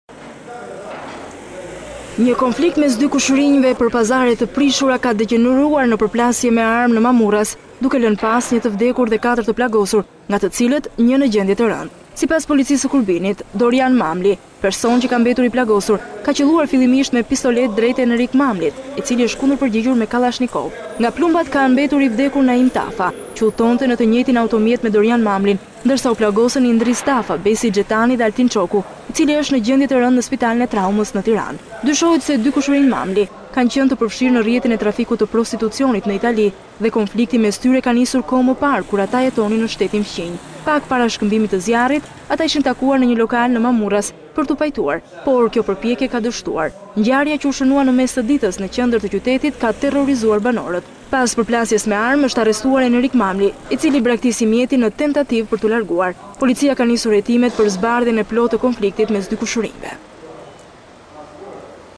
Mamurras, Dy kushërinjtë përplasen për rrjetin e prostitucionit, 1 i vrarë, 4 plagosen
Një person ka mbetur i vdekur dhe 4 të plagosur është bilanci i një përplasje me armë në Mamurras, ku protagonistë kanë qenë dy kushërinj.
Sipas hetimeve të para, sherri mes tyre ka nisur që në Itali kur ata dyshohet se ishin të përfshirë në rrjetin e prostitucionit. Dorian dhe Enrik Mamli tentuan sot të linin pas të shkuarën dhe të pajtoheshin, por në vend të shtërngimit të duarve, ata kanë shkëmbyer plumba.
Një konflikt mes dy kushërinjëve për pazare të prishura në Mamurras ka degjeneruar në përplasje me armë, duke lënë pas 1 të vdekur dhe 4 të plagosur, nga të cilët një në gjendje të rëndë.
Sipas policisë së Kurbinit, Dorian Mamli, personi që ka mbetur i plagosur, ka qëlluar fillimisht me pistoletë drejt Enrik Mamlit, i cili i është kundërpërgjigjur me kallashnikov.
Nga plumbat kanë mbetur i vdekur Naim Tafa që udhëtonte në të njëjtin automjet me Dorian Mamlin, ndërsa u plagosën Indriz Tafa, Besi Xhetani dhe Altin Çoku, i cili është në gjendje të rëndë në Spitalin e Traumës.
Dyshohet se dy kushërinjtë Mamli kanë qenë të përfshirë në rrjetin e trafikut të prostitucionit në Itali, dhe konflikti mes tyre ka nisur kohë më parë, kur ata jetonin në shtetin fqinj.
Pak para shkëmbimit të zjarrit ata ishin takuar në një lokal të Mamurrasit për t’u pajtuar, por kjo përpjekje ka dështuar.
Ngajrja që u shënua në mes të ditës në qendër të qytetit, ka terrorizuar banorët.
Pas përplasjes me armë, policia ka arrestuar Enrik Mamlin, i cili braktisi mjetin në tentativë për t’u larguar. Policia ka nisur hetimet për zbardhjen e plotë të konfliktit mes dy kushërinjve.